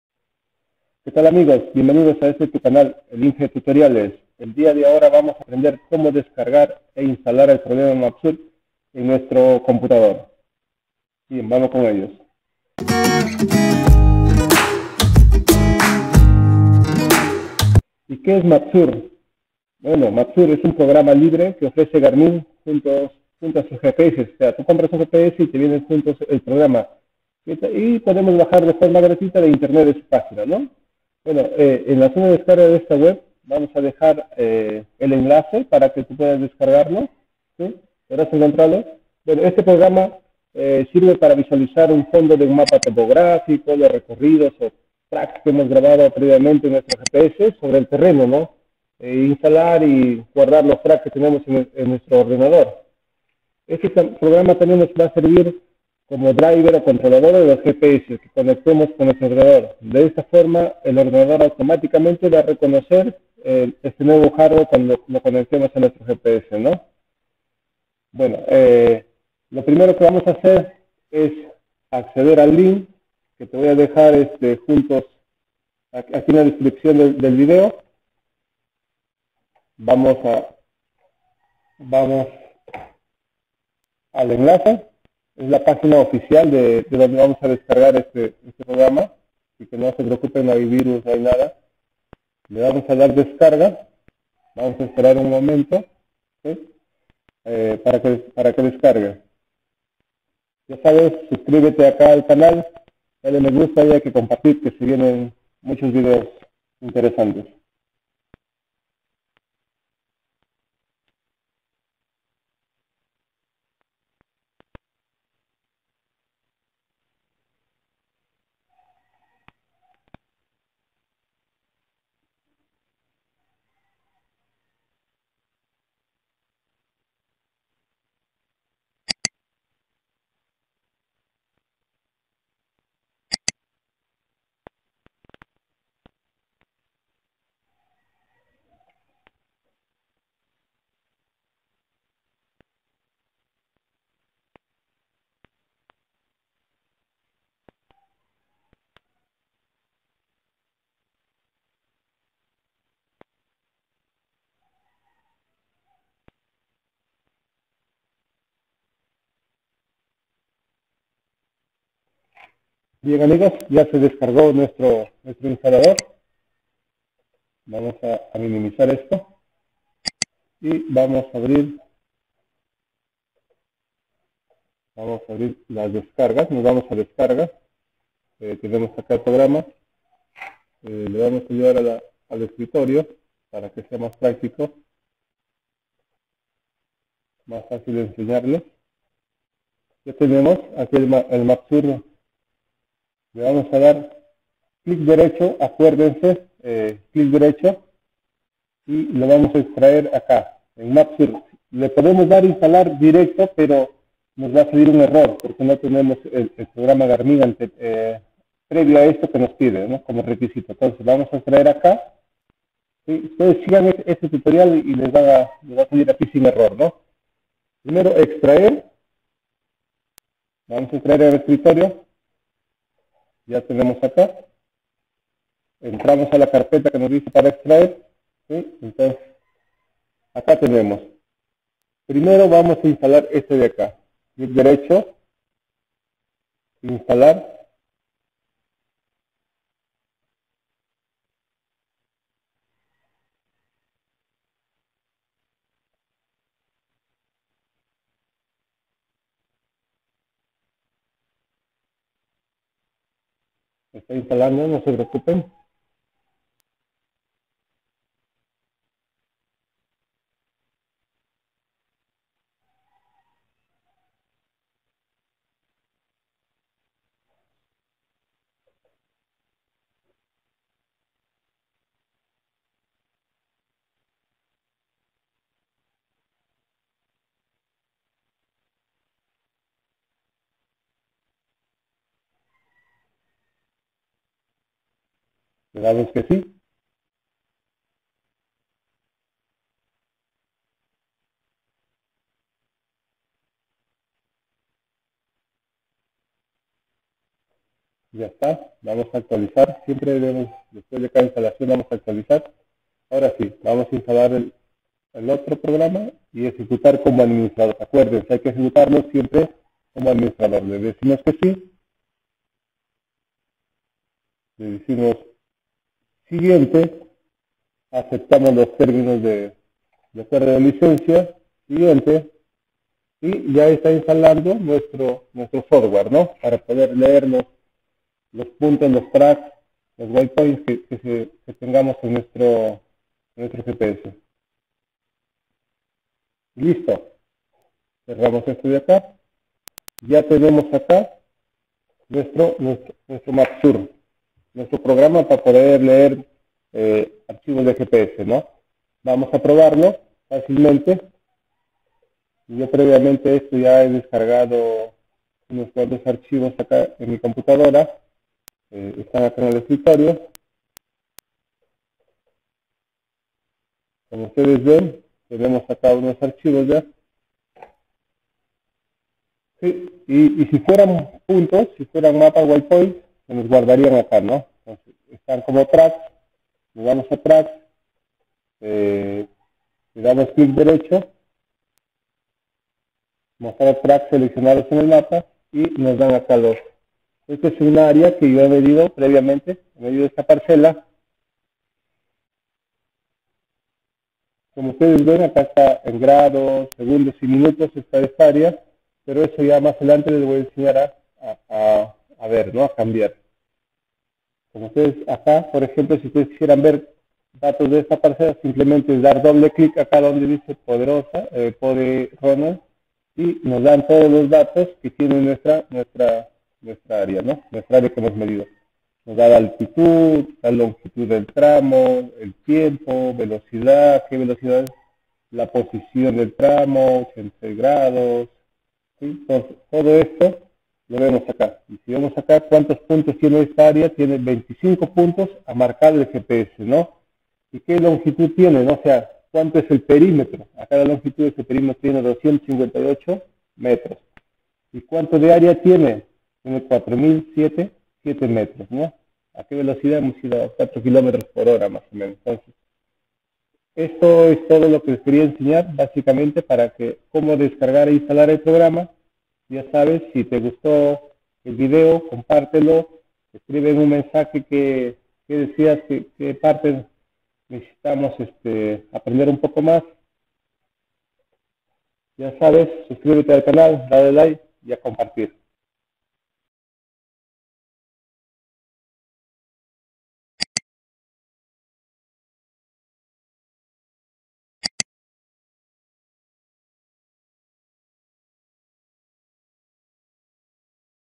Como descargar e instalar Mapsource para leer puntos de GPS
Aprende de forma fácil Como descargar e instalar Mapsource para leer puntos de GPS.